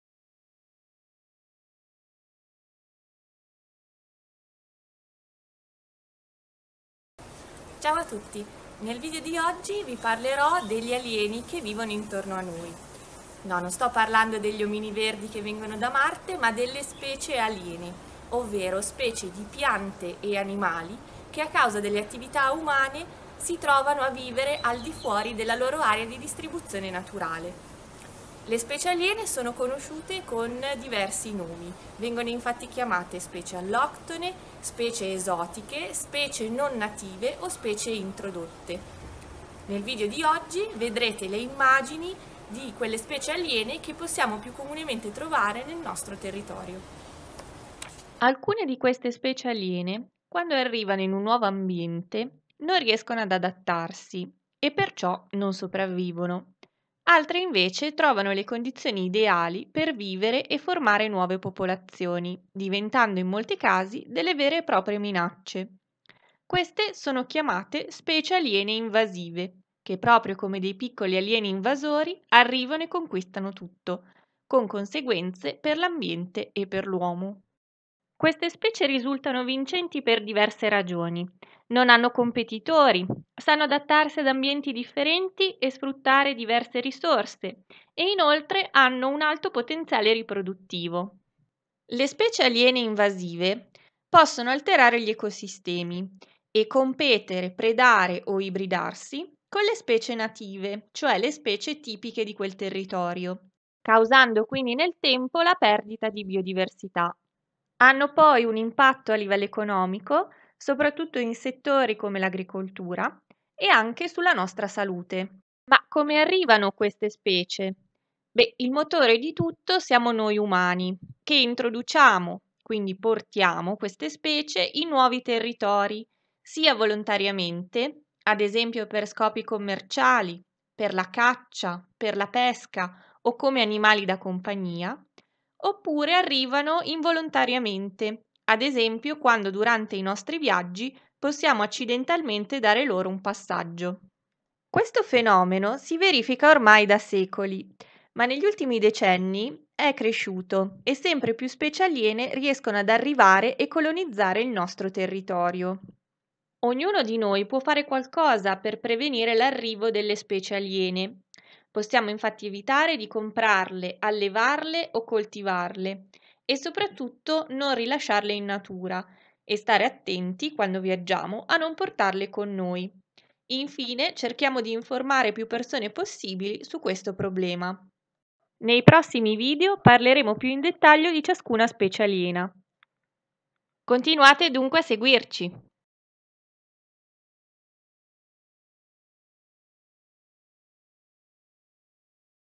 EvadicolParcoTicino LUNEDì Alieni tra noi
EvadicolParcoTicino. Oggi Emma ci parlerà delle specie aliene invasive , chiamate anche alloctone o esotiche, animali o vegetali, che arrivano e nel nostro habitat alterando gli ecosistemi